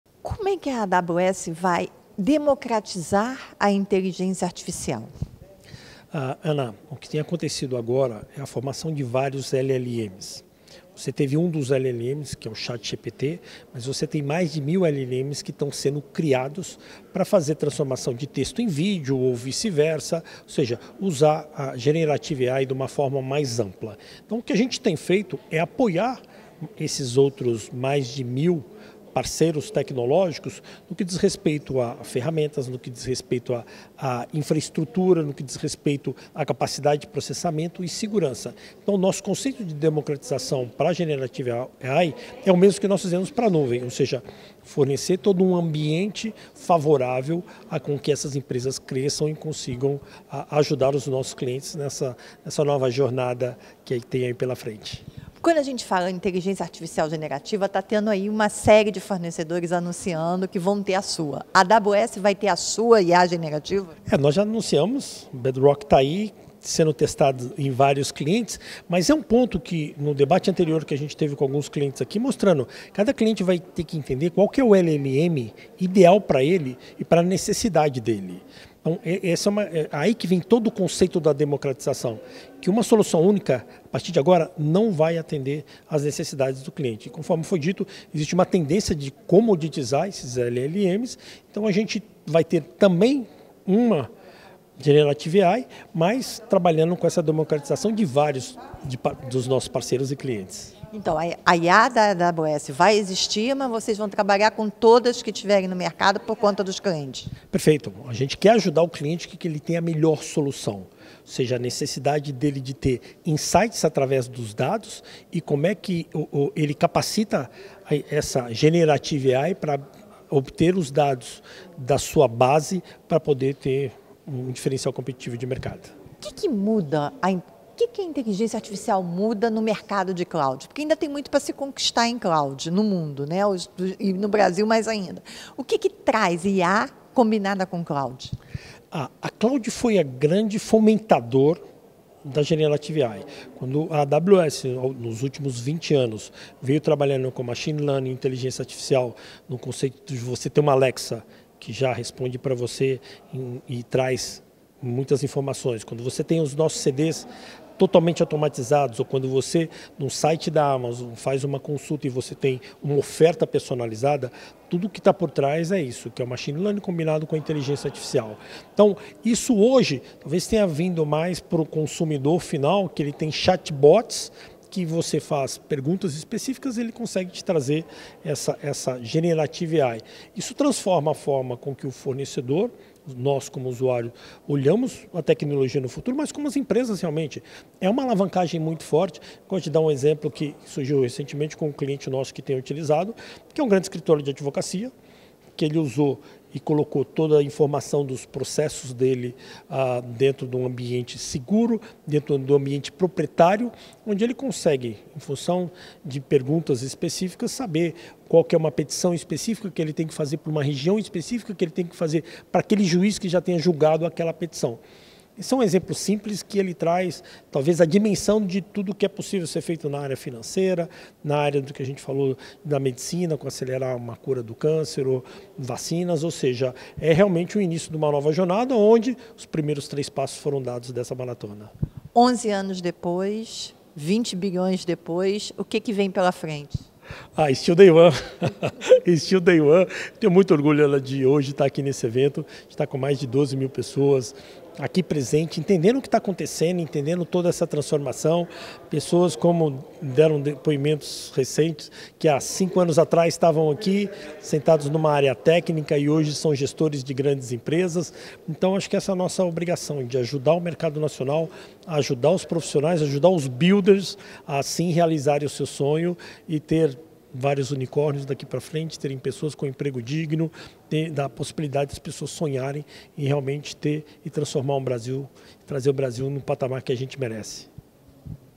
AWS investiu R$ 19,2 bilhões no Brasil
Diretor geral da AWS Brasil, Cleber Morais, revela que a AWS terá a sua IA generativa, a Bedrock, mas vai trabalhar de forma agnóstica com todas do mercado. "A AWS quer atender ao cliente. IA é para fazer negócio", afirmou.